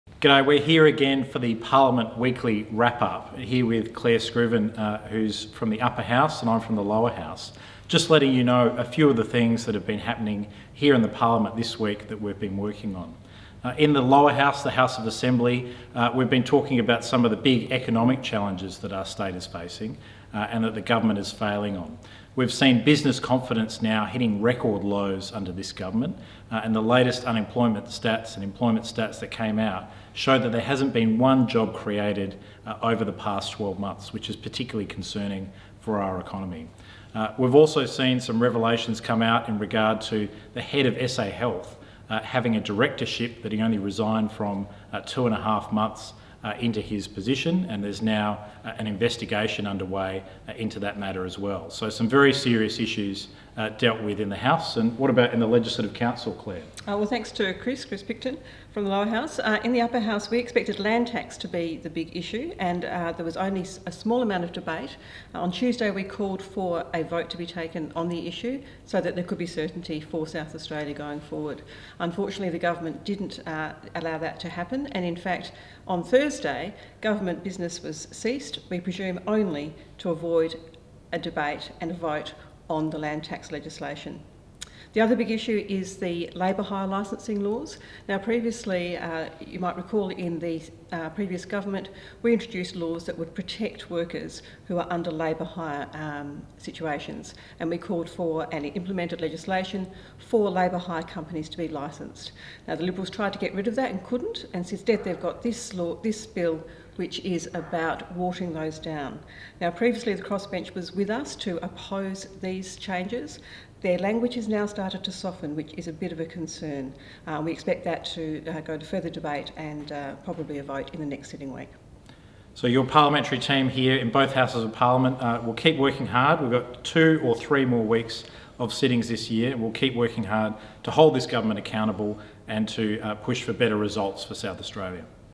Parliament Wrap Up November 15
The weekly wrap up of State Parliament with Chris Picton MP and Clare Scriven MLC.